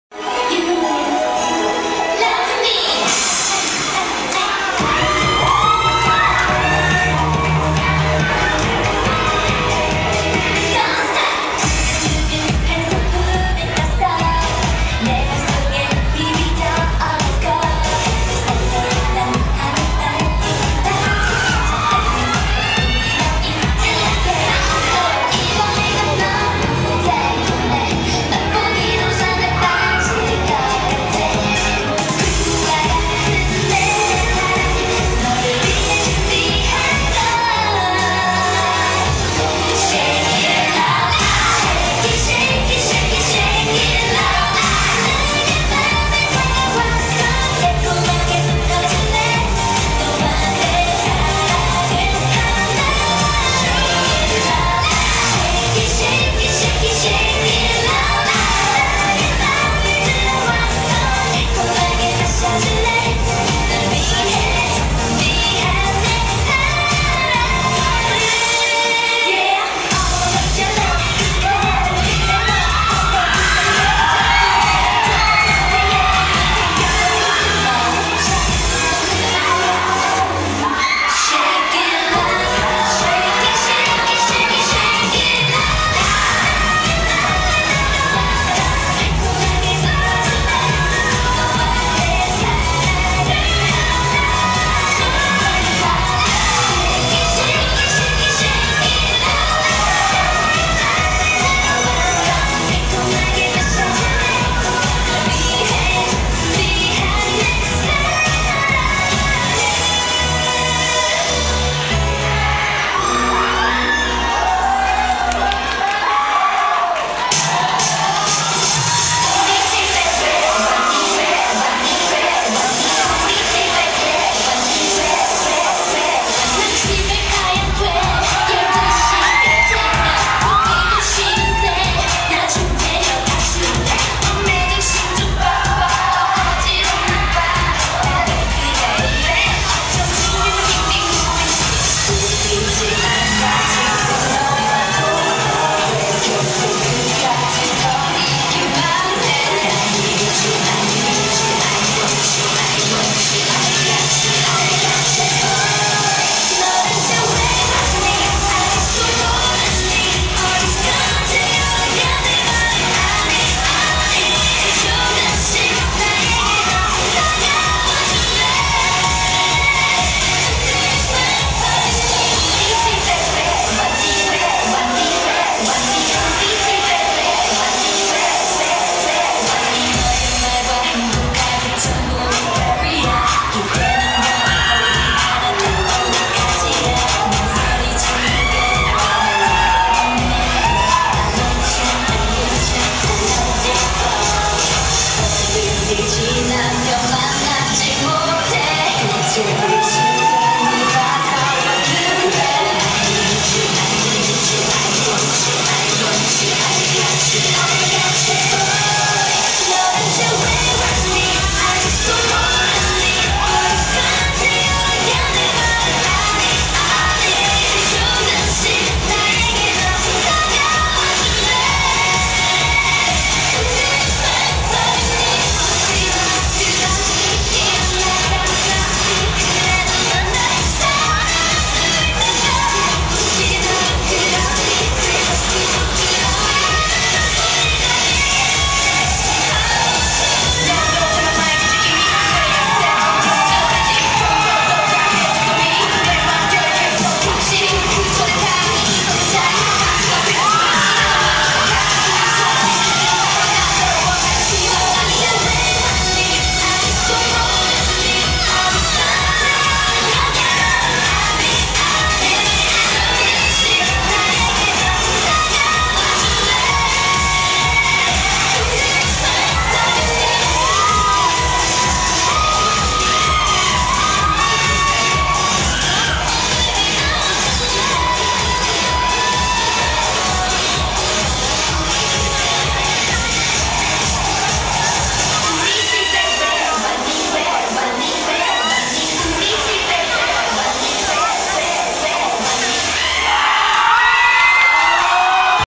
Event: Anime & K-Pop Clash - K-Pop Cover Dances 13
Mobile video of the K-Pop cover dances in Anime & K-Pop Clash, Walter Mart. San Fernando Pampanga, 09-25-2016

Video by Ilya Ashley using Asus Zenfone 2